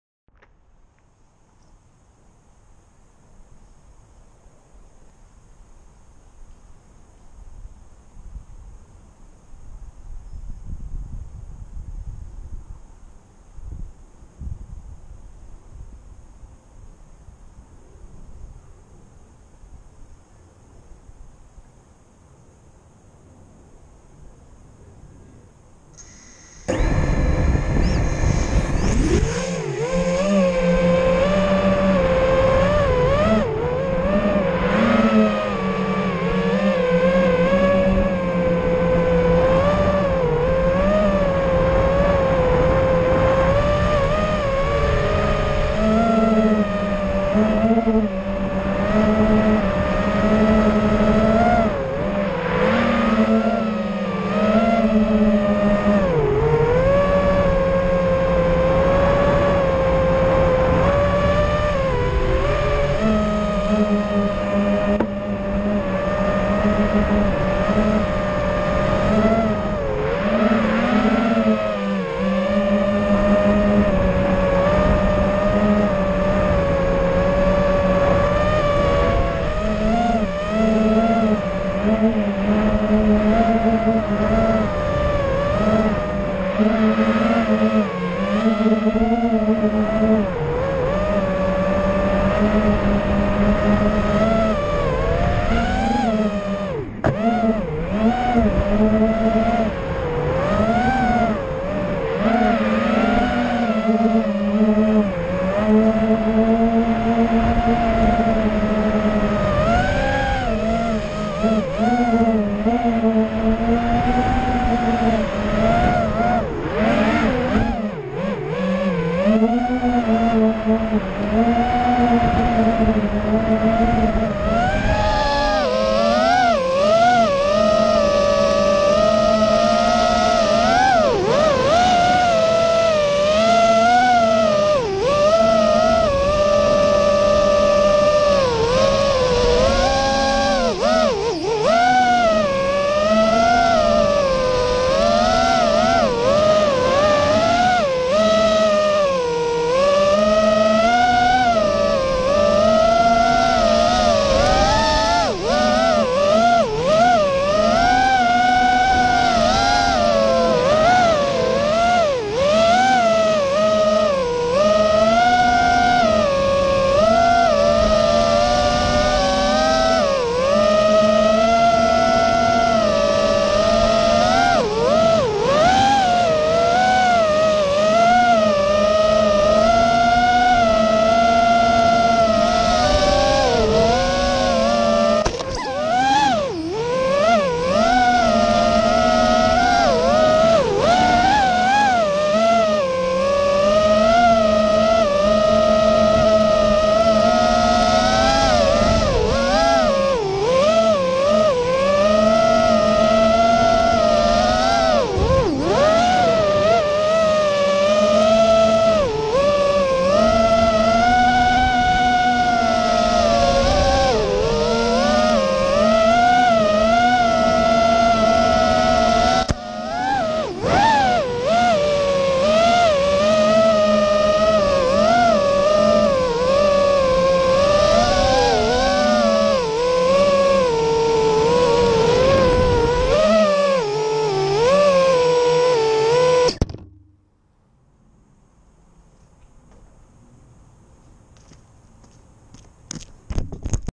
AstroX Maiden @ POW
Testing the new build racing with my buddy...this thing is fast! Barely using 3/4 throttle at times. Running Betaflight 3.5 and it's smooth as silk!

AstroX Silky Frame
Brother Hobby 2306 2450kv R6
Hobbywing 45a 4in1
Hobbywing G2 FC
R-Xsr
Axxi Stubby
Matek LED's
Swift V2 Cam

3d Printed Bits:

Front Bumper
Stubby Mount
Arm Guards
GoPro Mount